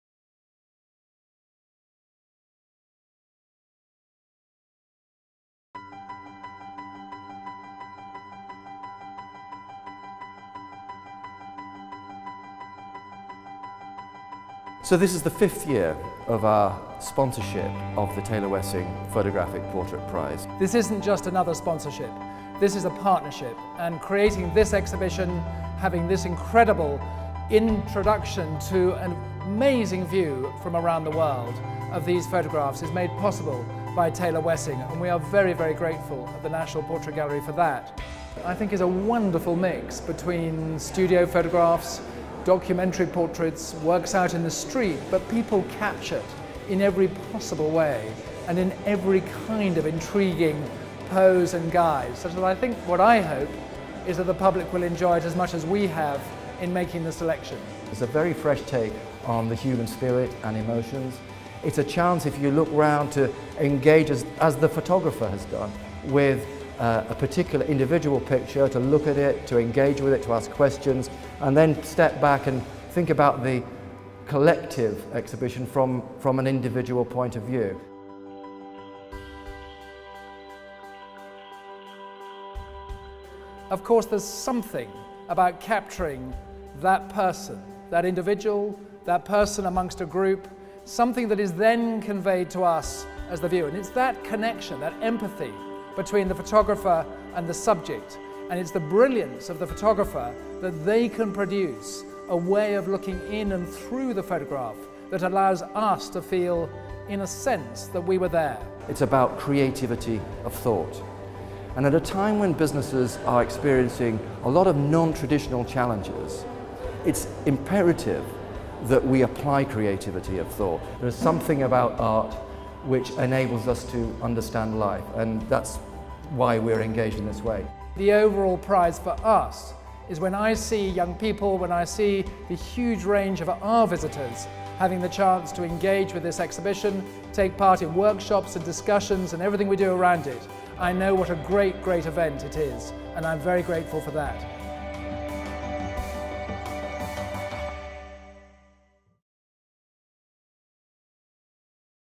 National Portrait Gallery Photographic Portrait Prize 2012
The Taylor Wessing Photographic Portrait Prize 2012 offers a unique opportunity to see sixty new portraits by some of the most exciting contemporary photographers from around the world.

This short video shows highlights of the exhibition together with speeches from Sandy Nairne, the Director of National Portrait Gallery and Tim Eyles, Managing Partner of Taylor Wessing LLP.

Recorded by BRR Media